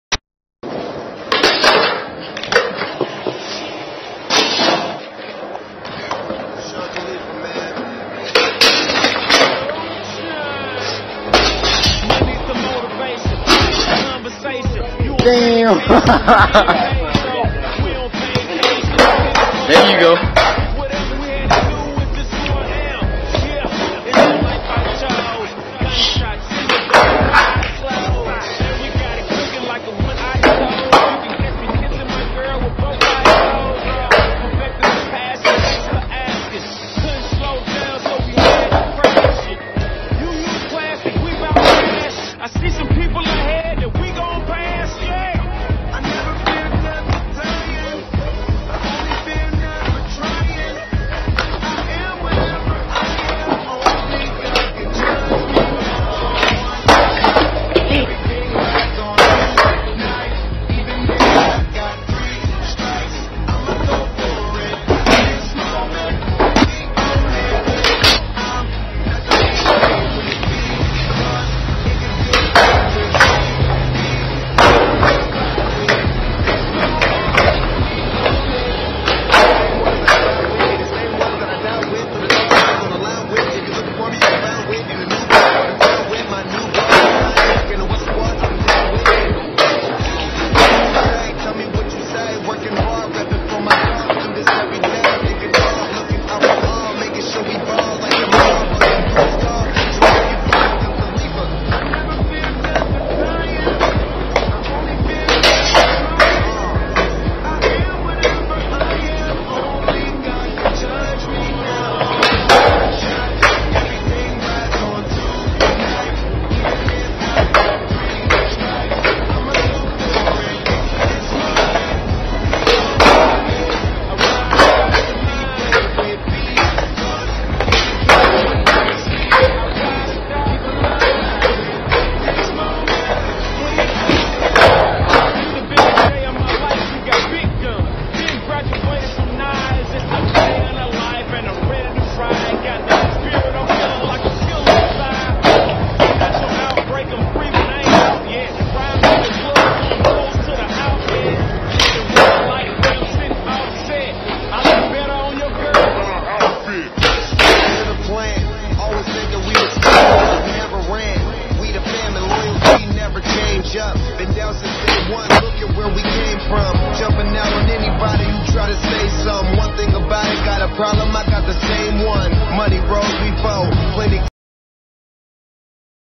The new and improved
Me and my boi chris going at it again, showing off new tricks, and having a good time doing what we do best!